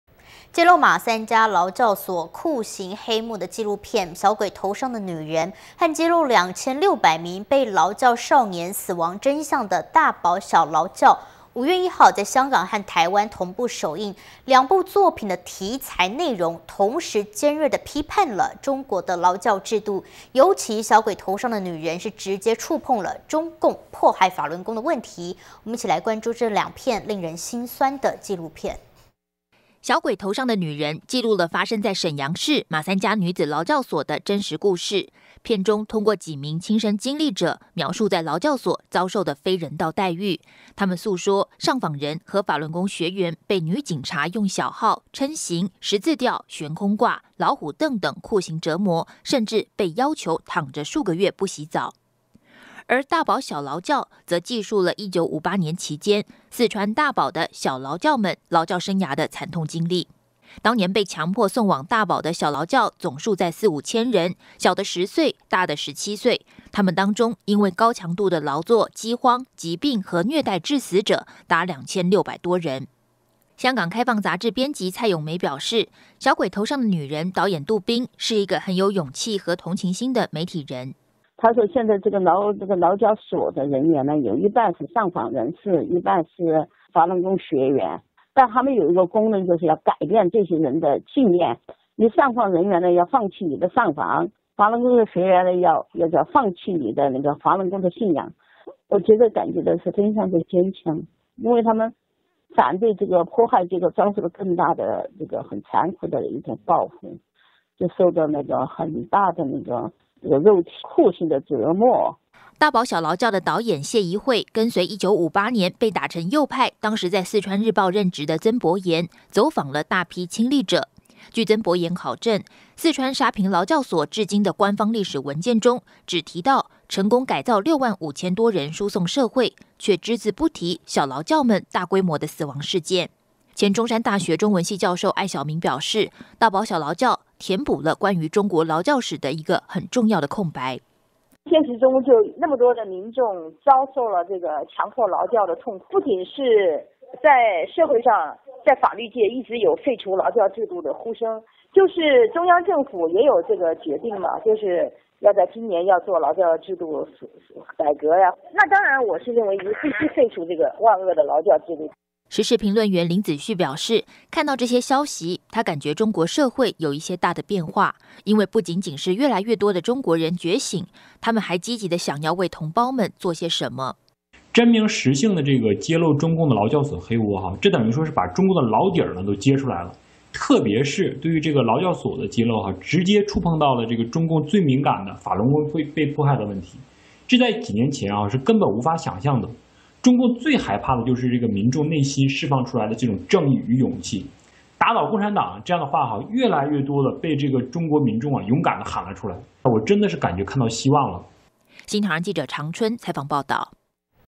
【馬三家勞教所_中國真相最新新聞】[禁聞]勞教制度下的婦孺 兩紀錄片首映
【中國真相新聞】(China Breaking News)兩部作品的題材、內容，同時尖銳的批判了中國的勞教制度，尤其《小鬼頭上的女人》，直接觸碰了中共迫害法輪功問題。一起來關注這兩部令人心酸的紀錄片。【新唐人亞太台2013年5月1日訊】

《小鬼頭上的女人》，記錄了發生在瀋陽市「馬三家女子勞教所」的真實故事。片中通過幾名親身經歷者描述在勞教所遭受的非人道待遇。她們訴說﹕「上訪人和法輪功學員被女警察用小號、抻刑、十字吊、懸空掛、老虎凳等酷刑折磨，甚至被要求躺著數個月不洗澡。」

而《大堡小勞教》則記述了1958年期間，四川大堡的「小勞教」們勞教生涯的慘痛經歷。

當年被強迫送往大堡的「小勞教」總數在四、五千人，小的十歲，大的十七歲，他們當中因為高強度的勞作、飢荒、疾病和虐待等致死者，達2600人之多。

香港《開放》雜誌編輯蔡詠梅表示，《小鬼頭上的女人》導演杜斌，是一個很有勇氣和同情心的媒體人。

香港《開放》雜誌執行主編 蔡詠梅：「他（杜斌）說現在勞教所裏面的人員一半是上訪人員，一半是法輪功學員，它（勞教所）有個功能是改變這些人的信念，上訪人員要放棄上訪，法輪功學員要放棄你的法輪功的信仰。我覺得她們是非常的堅強，她們反對迫害遭受這種很殘酷的報復，受到很大的肉體酷刑的折磨。」

《大堡小勞教》的導演謝貽卉，跟隨1958年被打成右派、當時在《四川日報》任職的曾伯炎，走訪了大批親歷者。據曾伯炎考證，「四川沙坪勞教所」至今的官方歷史文件中，僅提到﹕成功改造六萬五千多人輸送社會，卻隻字不提「小勞教」們大規模死亡事件。

前中山大學中文系教授艾曉明表示，《大堡小勞教》填補了關於中國勞教史的一個很重要的空白。

前中山大學中文系教授 艾曉明：「現實中那麼多的民眾遭受了這個強迫勞教的痛苦。不僅在社會上，在法律界，一直有廢除勞教制度的呼聲，就是中央政府也有這個決定，就是要在今年要做勞教制度的改革。那當然，我是主張廢除這個萬惡的勞教制度。」

時事評論員林子旭表示，看到這些消息，他感覺中國社會有一些大的變化，因為不僅僅是越來越多的中國人覺醒了，他們還在積極的想要為同胞做些什麼。

時事評論員 林子旭：「真名實姓的揭露中共的勞教所黑窩，這等於是把老中共的老底兒都掀出來了，特別是對於勞教所的揭露，直接觸碰到了中共最敏感的法輪功被迫害的問題，這在幾年前是根本無法想像的，中共最害怕的就是民眾內心釋放出來的正義與勇氣，打倒共產黨這樣的話，越來越多的被中國人勇敢的喊了出了，我真的感覺看到希望了。」